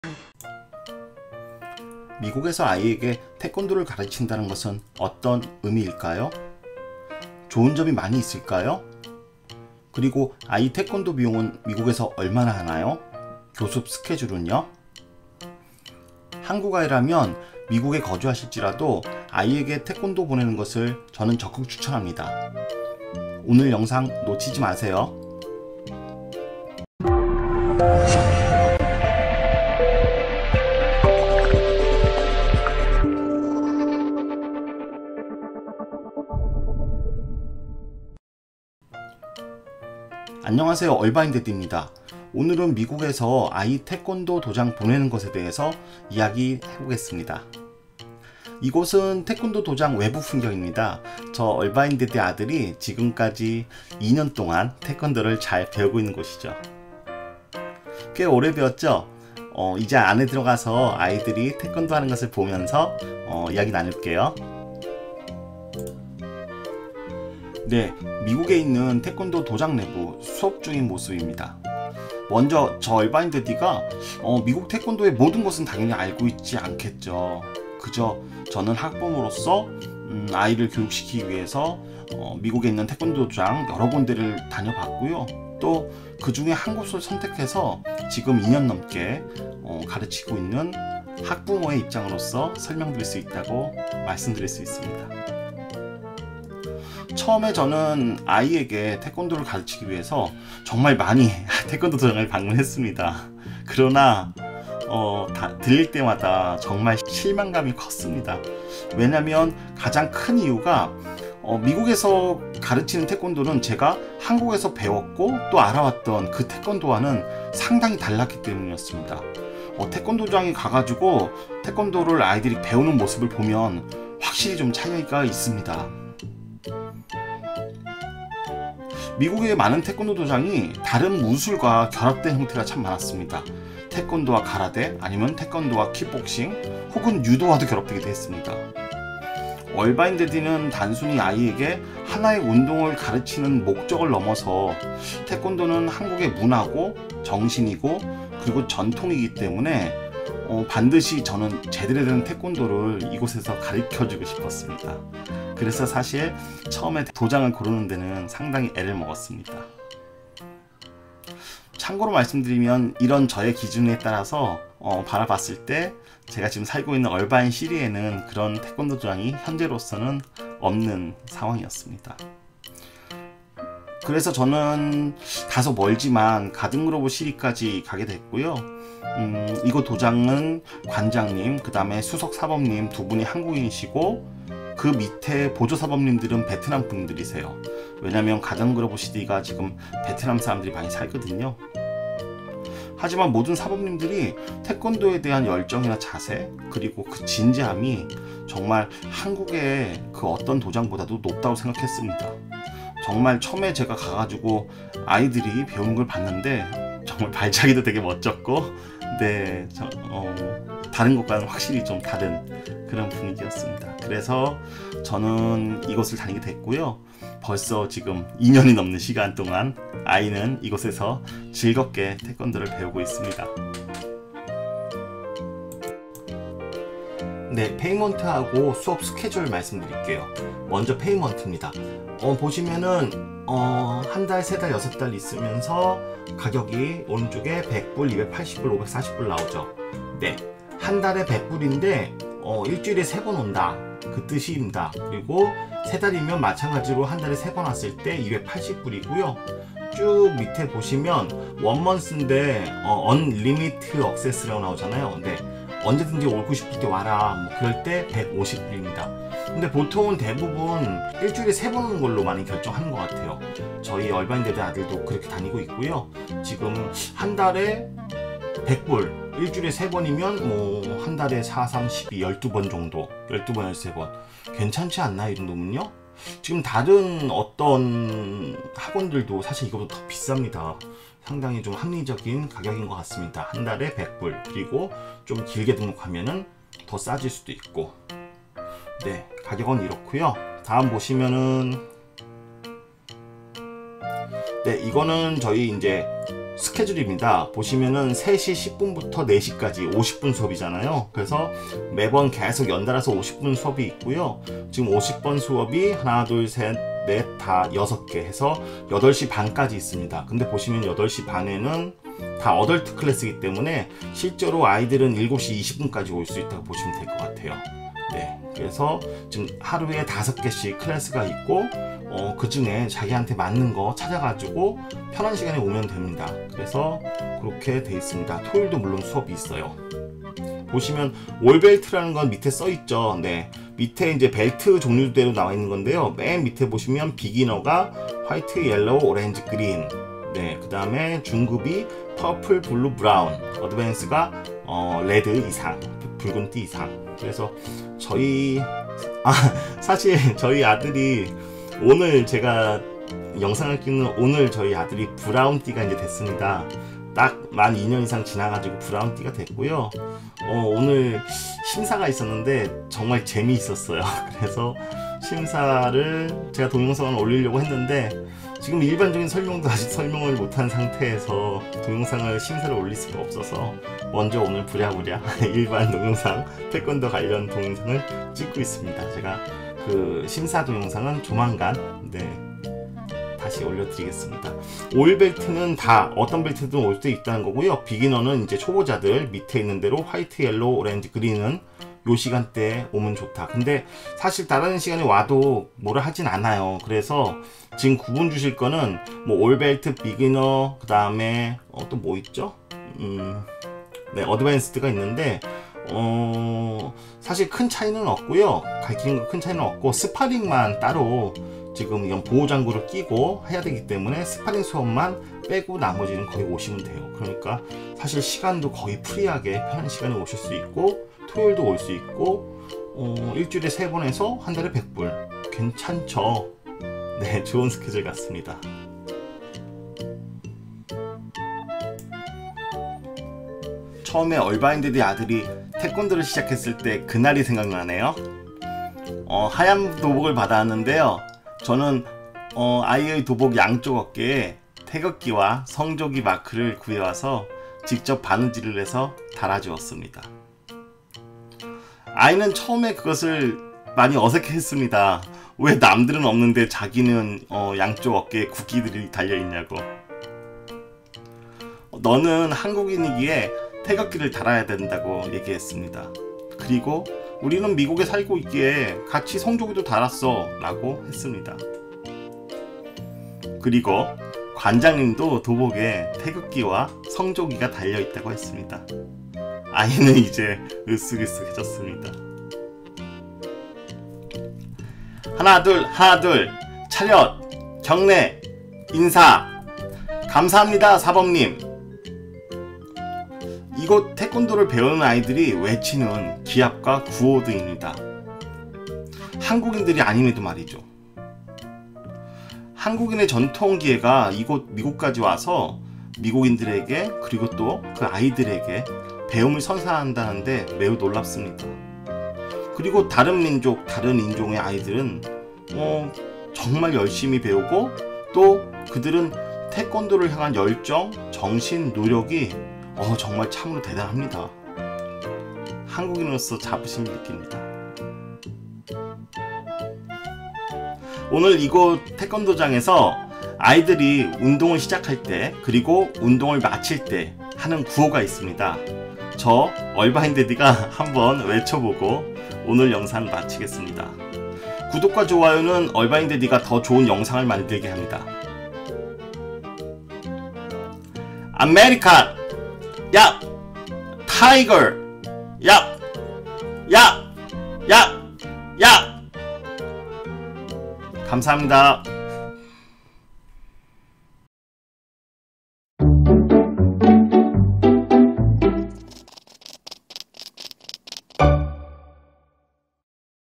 [얼바인대디] 미국에서 아이 태권도 가르치는 법 _ Tea Kwon Do
미국에서 아이를 태권도장에 보내는 것에 대한 이야기 입니다. 2년 넘게 태권도장을 보내면서 신체적으로나 정신적으로나아이가 많이 성장했습니다. 미국에서 태권도장을 어떻게 선택해야 하는지? 비용은 얼마인지? 어떻게 배워가는지에 대한 이야기를 합니다.